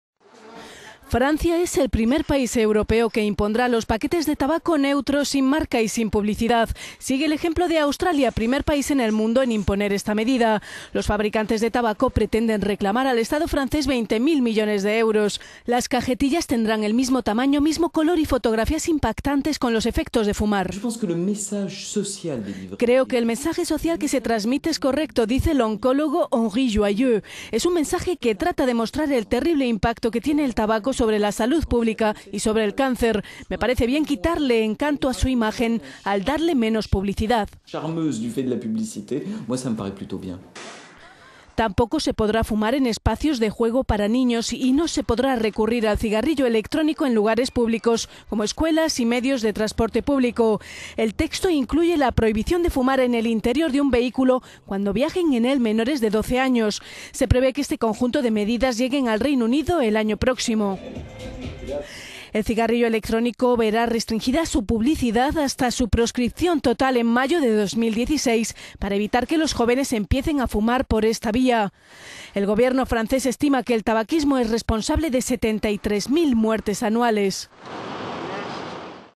Francia impondrá paquetes de tabaco sin marca y sin publicidad
Francia es el primer país europeo que impondrá los paquetes de tabaco "neutros", sin marca y sin publicidad. Sigue el ejemplo de Australia, primer país en el mundo en imponer esta medida.

Los fabricantes de tabaco pretenden reclamar al Estado francés 20.000 millones de euros.

Las cajetillas tendrá el mismo tamaño, el mismo color y fotografías impactantes con los efectos de fumar.

"Creo que el mensaje social que se transmite es correcto, dice el oncólogo Henri Joyeux. Es un mensaje que trata …